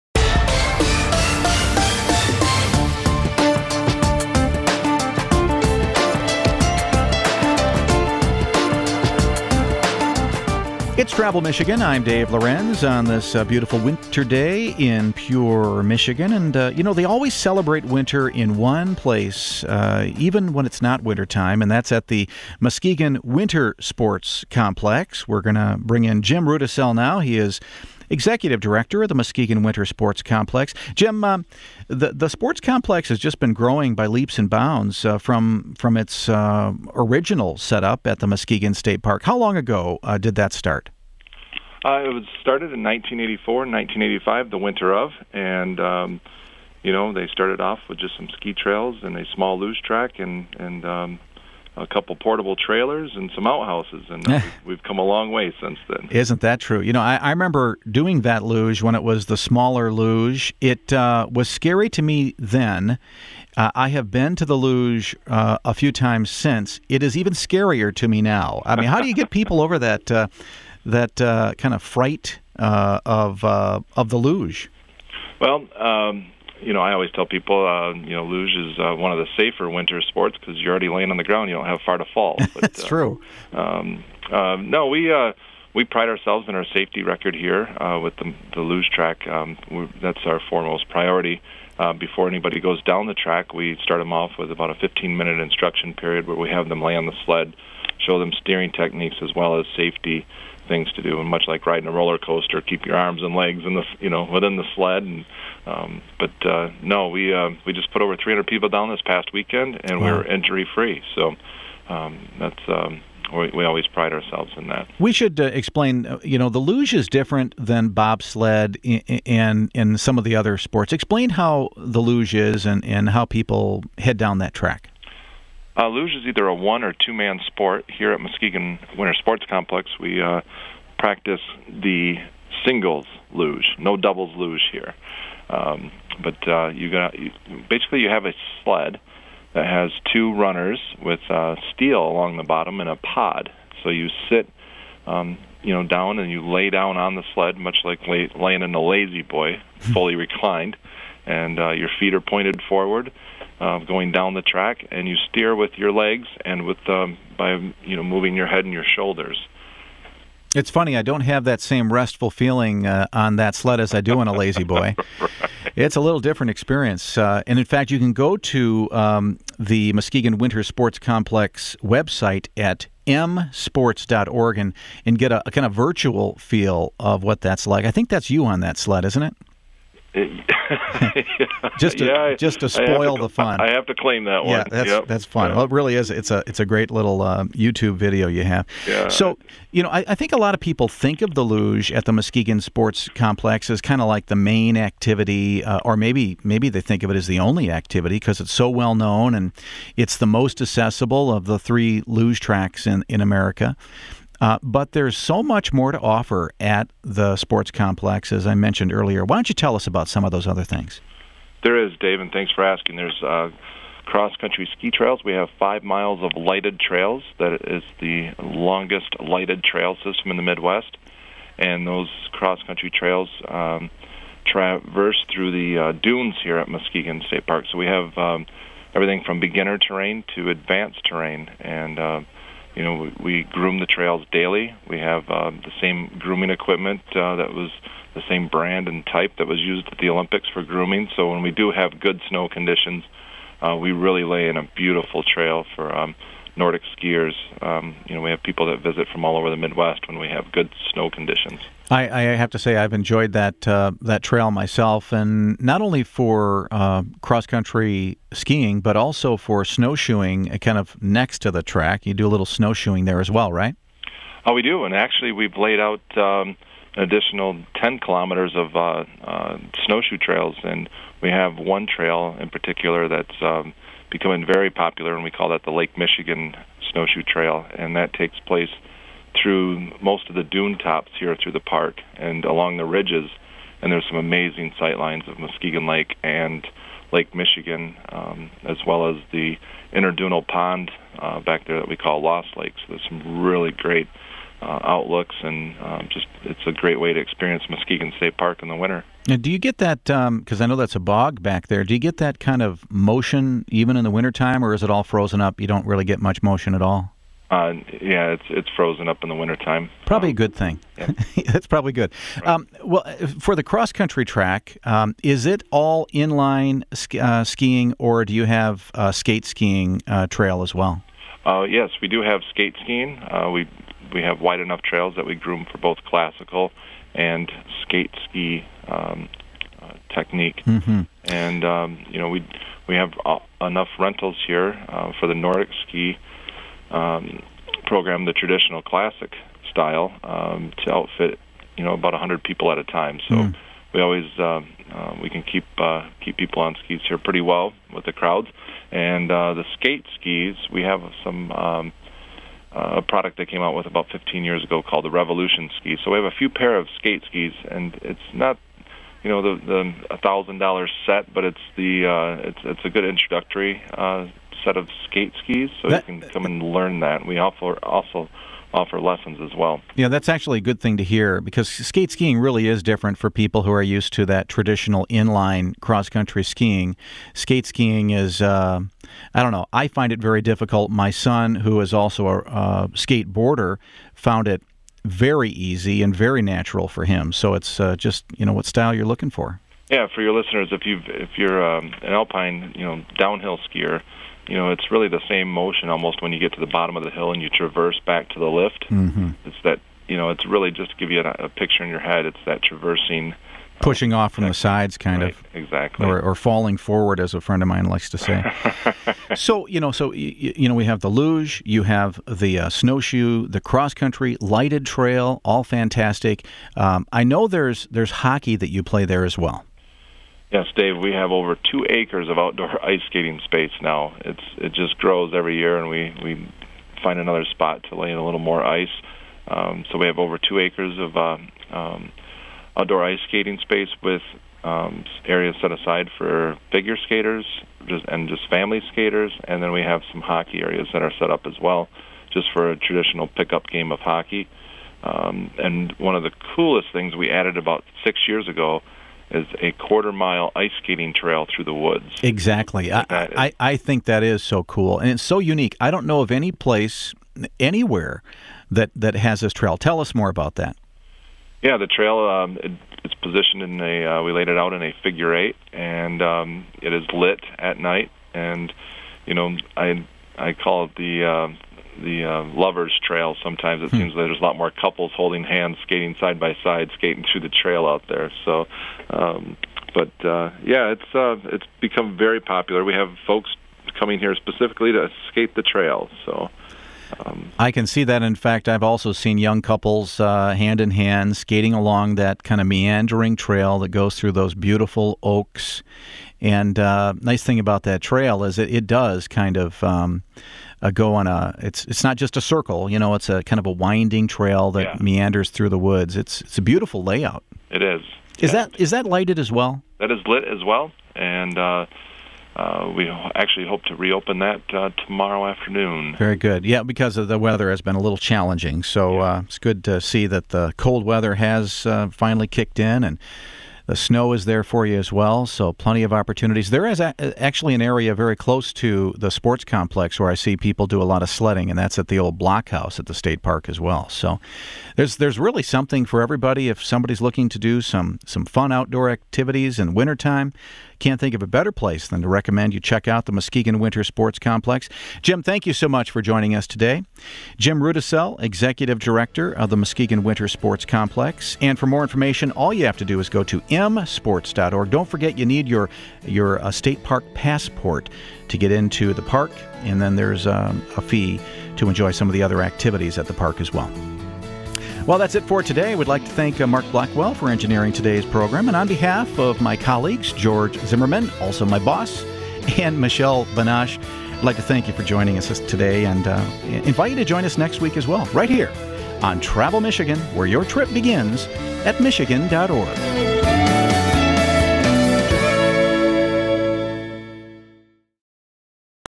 Muskegon Ice Luge Track Lessons | Pure Michigan
The luge experience takes place at the Muskegon Winter Sports Complex, which is one of only four luge tracks that exist in the United States. Shorter in overall length than the Olympic tracks located in New York and Salt Lake City, the Muskegon track provides an Olympic thrill with the safety of the participant in mind. Top speeds of approximately 30 mph are possible from the public start area located about 3/4 of the way up the track.